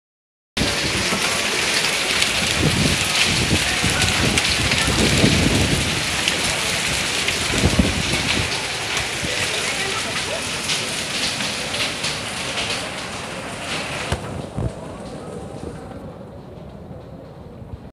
Fuerte granizada en la zona de Metro Puerta de Arganda en Madrid
Emergencias Madrid alertó de diversos problemas en distintos puntos de la capital destacando fuertes tormentas de granizo que han descargado enormes bolas de hielo sobre la calzada de varias zonas de la capital.